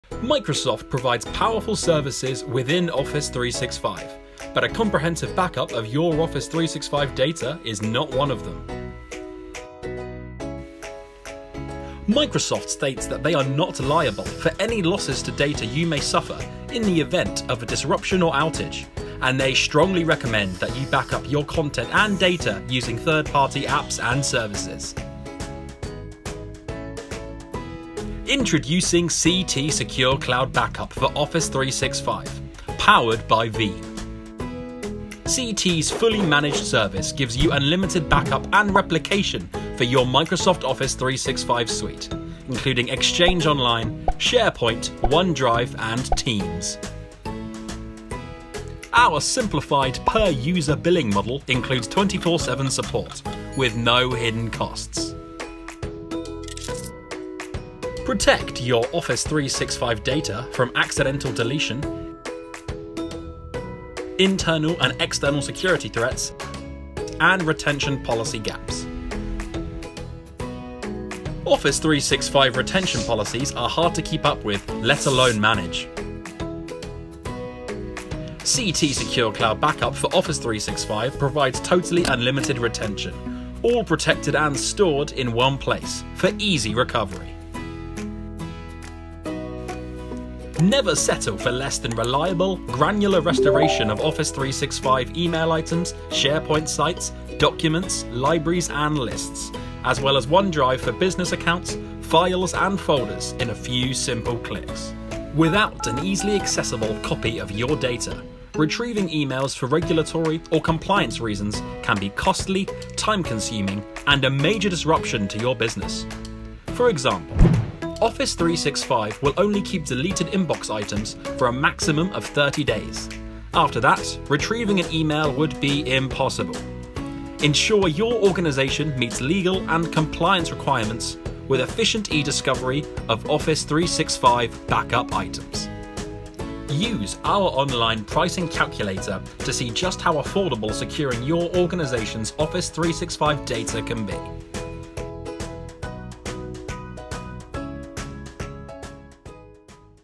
CT Secure Cloud Backup for Office 365
CT Secure Cloud Backup for Microsoft Office 365, powered by Veeam, is a solution that provides complete backup and recovery for your Office 365 Business/Enterprise data including Exchange Online, SharePoint Online and OneDrive for Business.